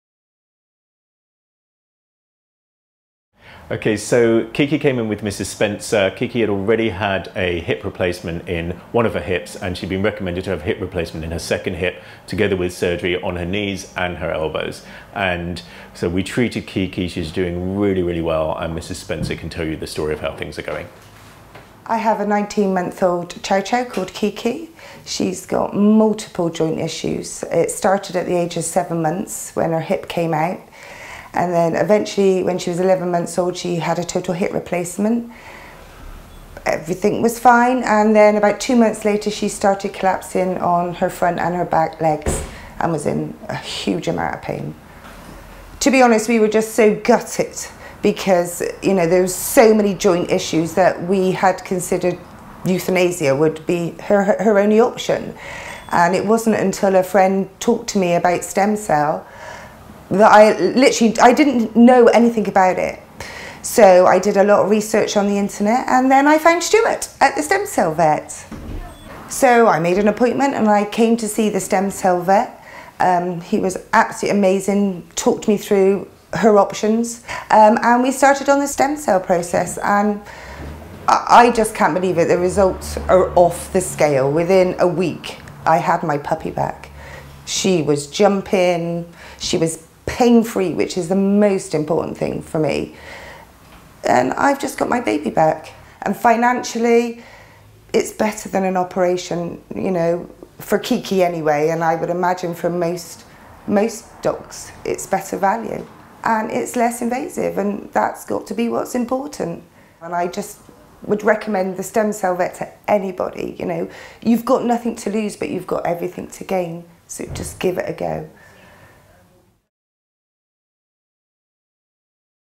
Stem Cell Vet review - treatment of arthritis in dogs
Pet Owner Mrs Spencer talks about how Stem Cell Vet helped her dog, Kiki, with arthritis using stem cell therapy.